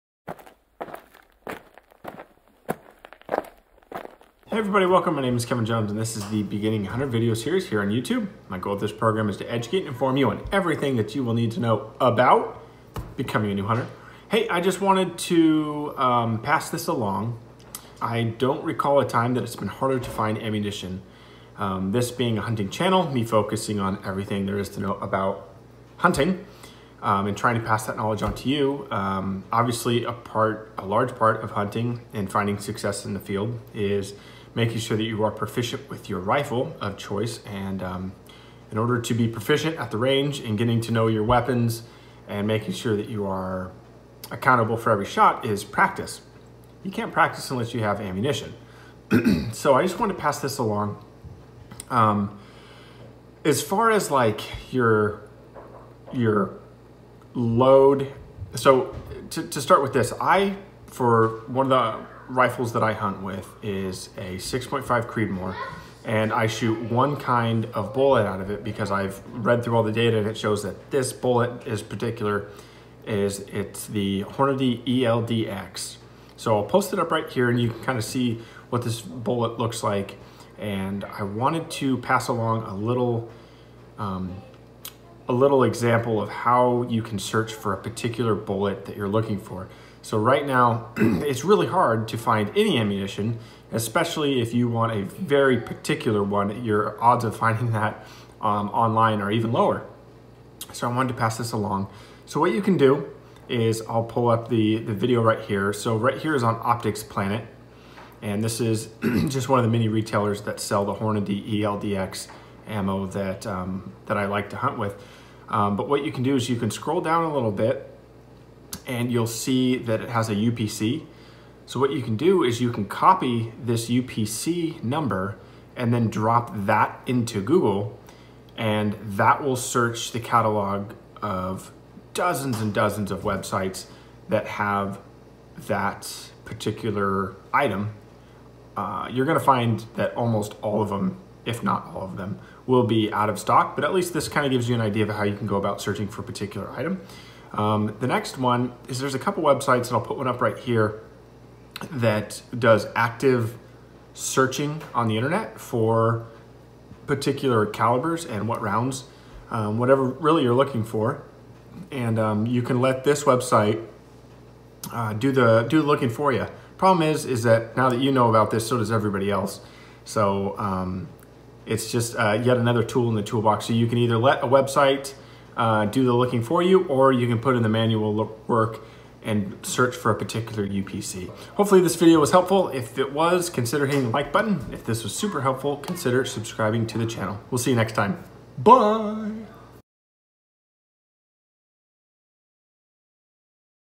2021: Two Best Ways to Find Ammo
There are two ways that you can search for ammunition for this upcoming hunting season. Find out how!